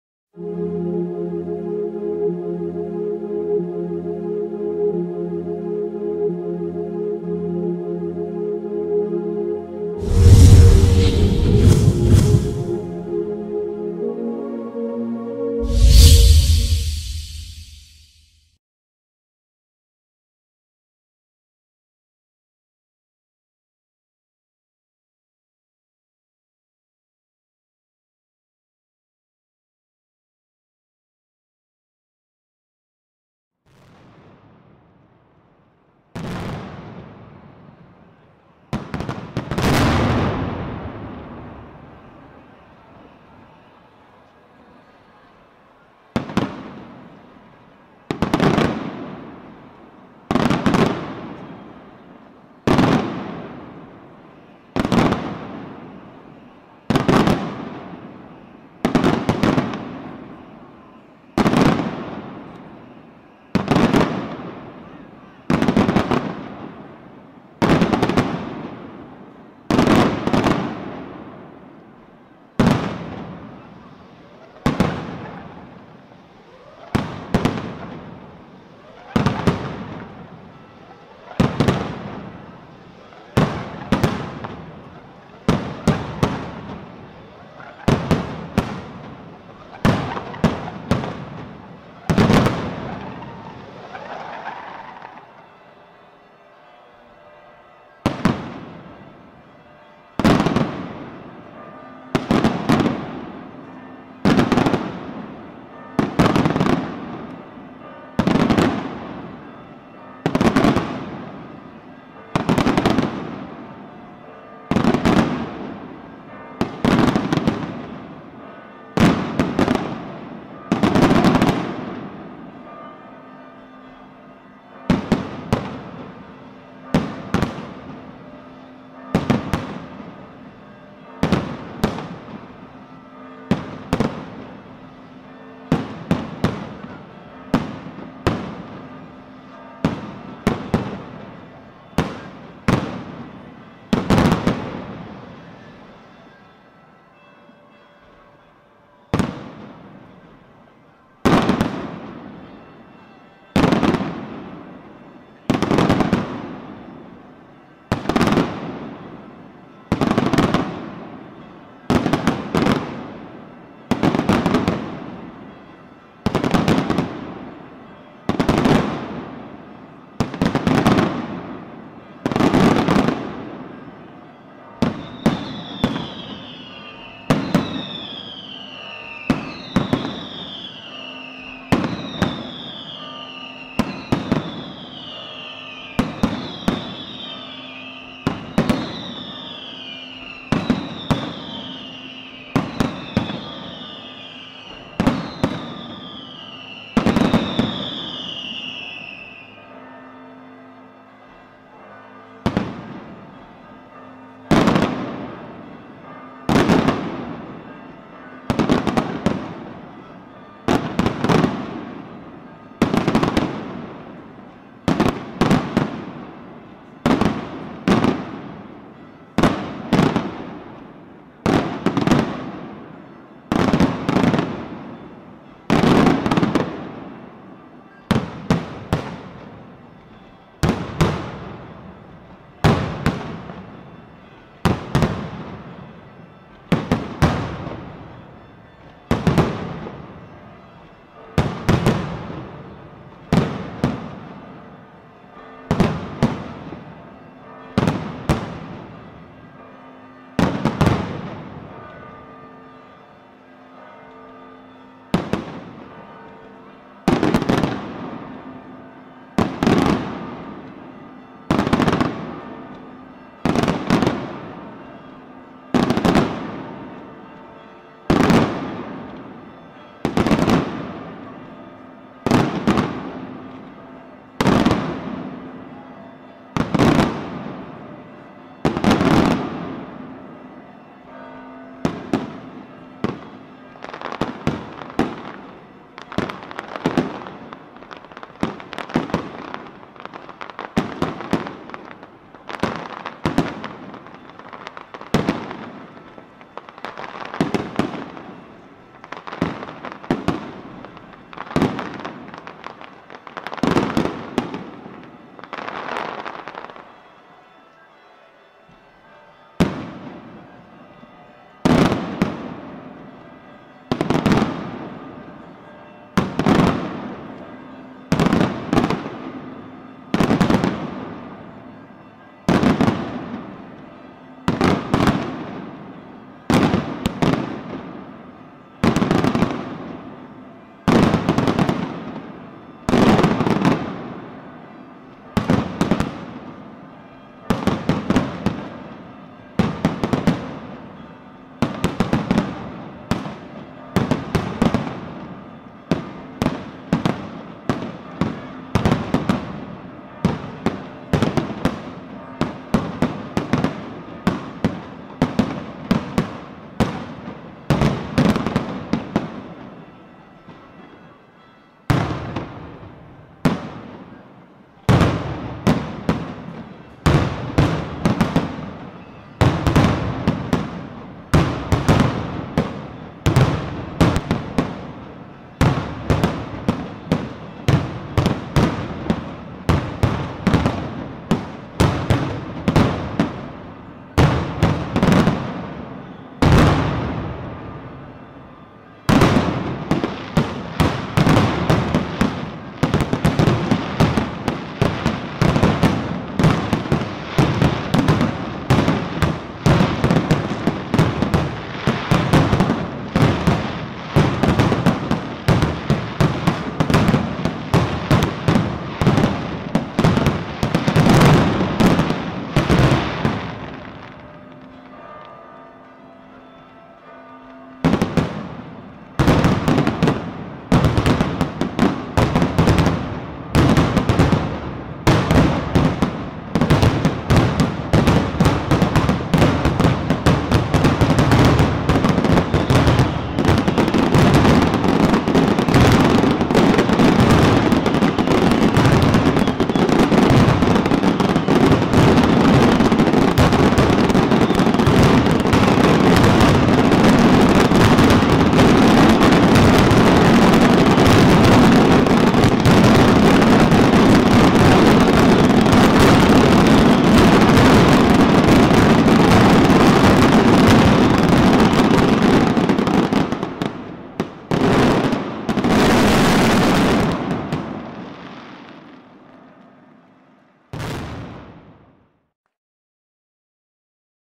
CATANIA - Sant'Agata 2019 - VACCALLUZZO Events (5 Febbraio Uscita)
CATANIA - Sant'Agata 2019 - 5 Febbraio

Tradizionale uscita a cura di
VACCALLUZZO Events da Belpasso (Ct)